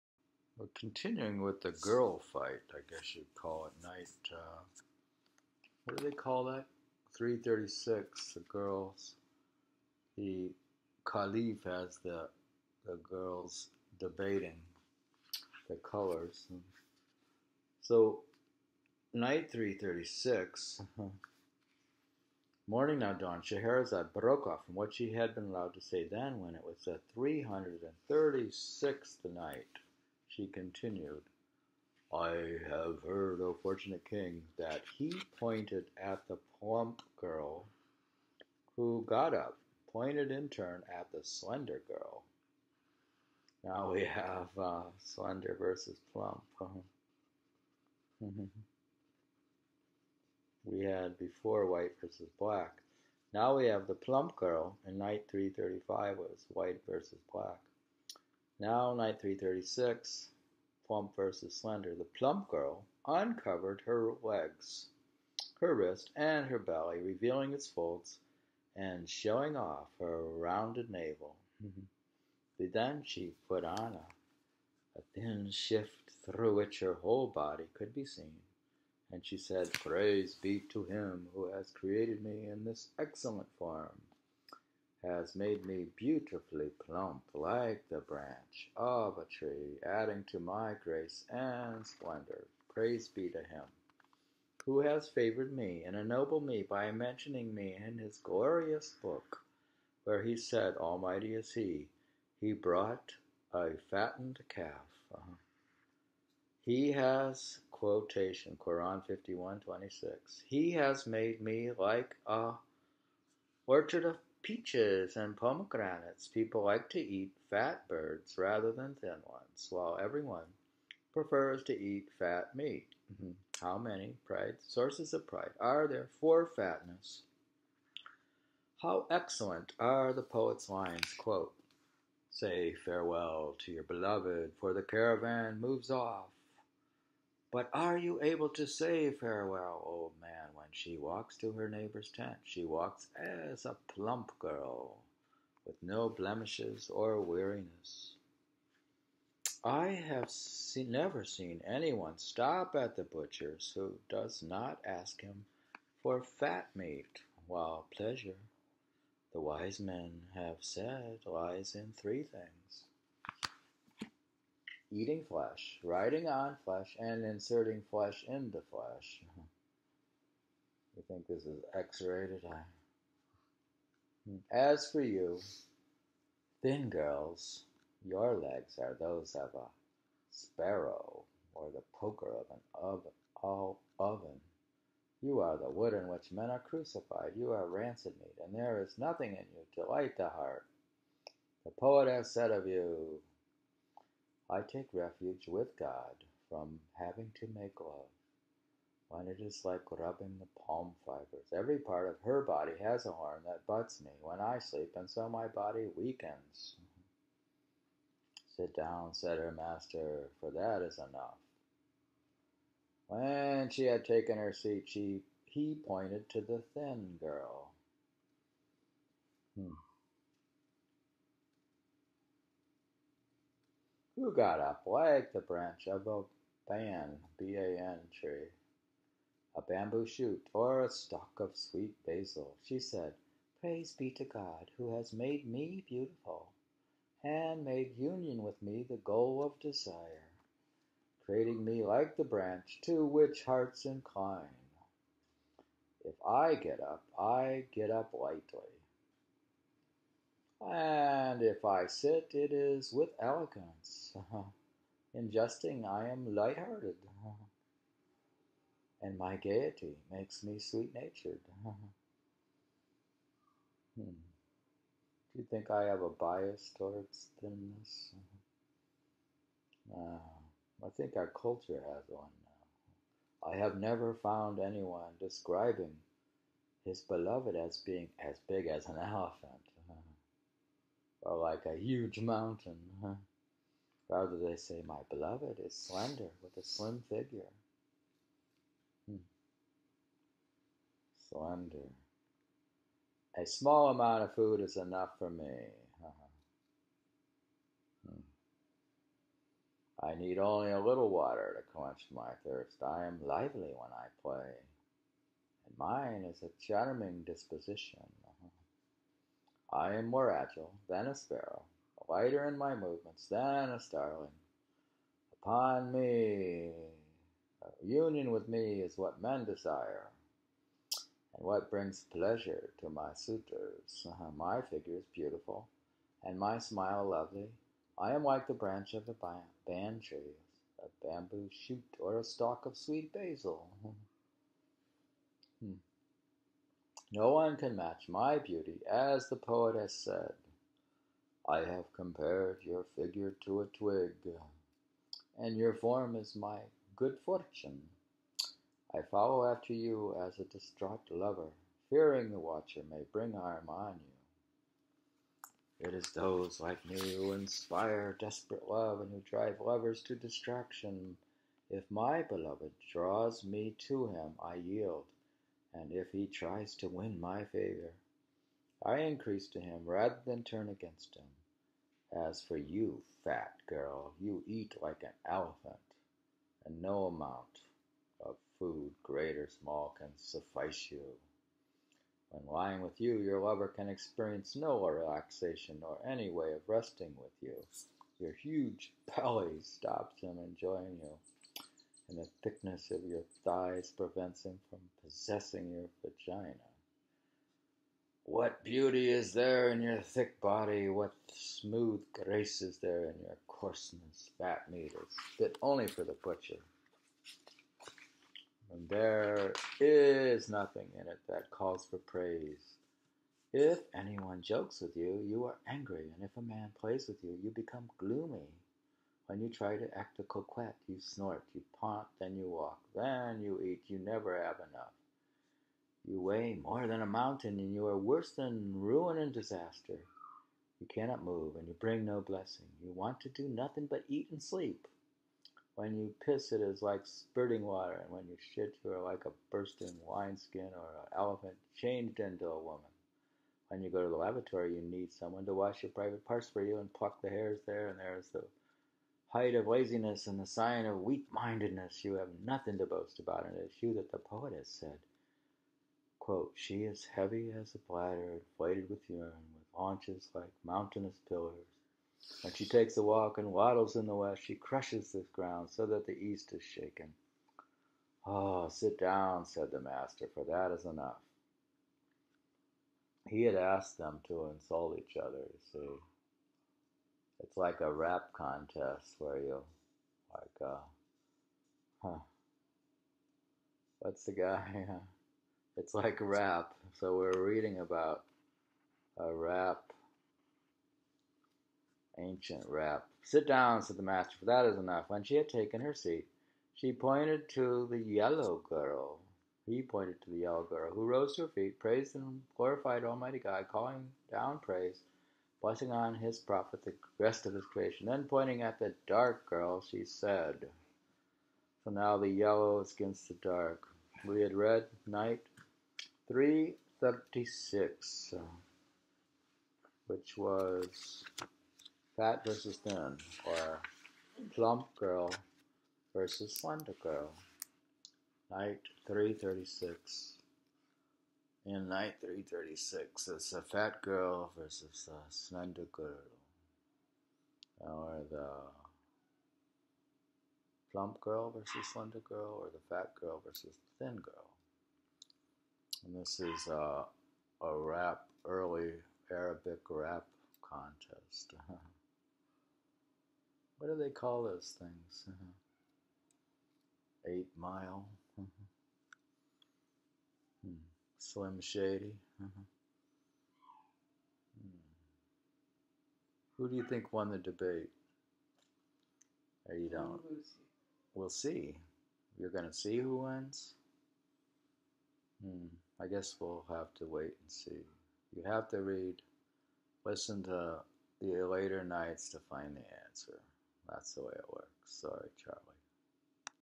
Arabian Nights night 336 fat girls versus thin girls
The Arabian Nights tales of 1001 nights translated by Malcolm Lyons and read by David Smith NYC davidsmith208 wix blog and vaapaaran instagram  plump girls rap debate contest with the slender girl tale of the six slave girls  8 mile or slim shady thousand and one nights night 336 debate between the white black plump slender yellow and dark girls